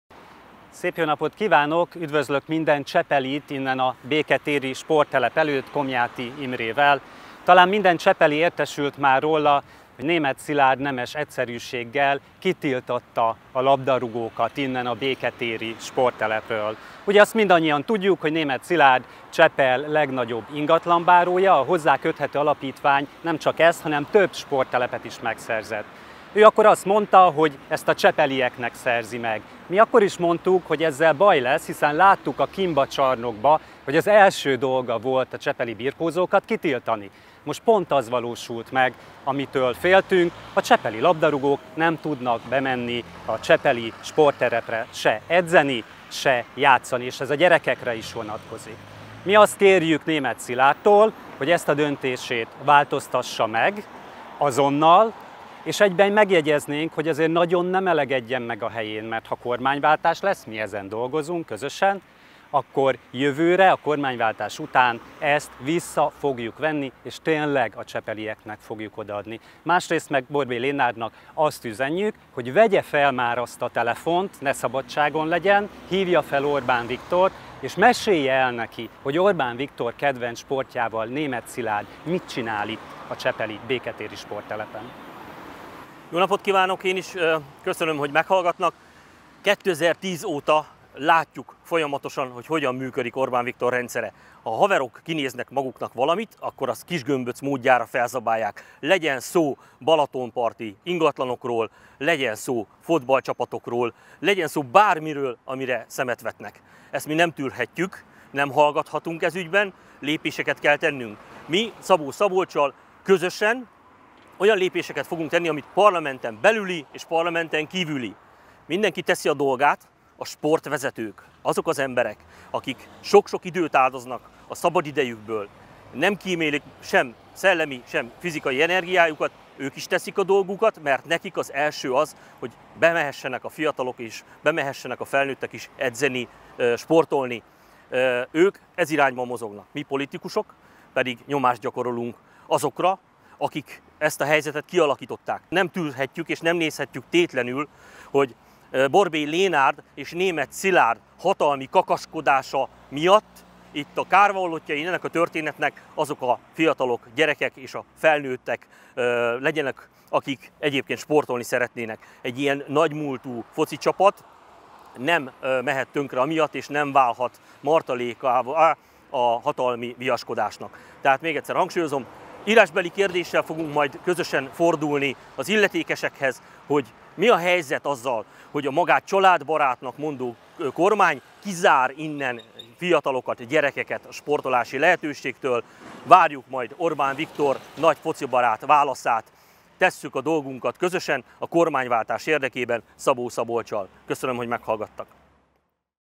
MSZP | Támogatjuk, hogy a sportoló emberek, családok visszakapják a csepeli sporttelepet!
Komjáthi-Szabó: Támogatjuk, hogy a sportoló emberek, családok visszakapják a csepeli sporttelepet!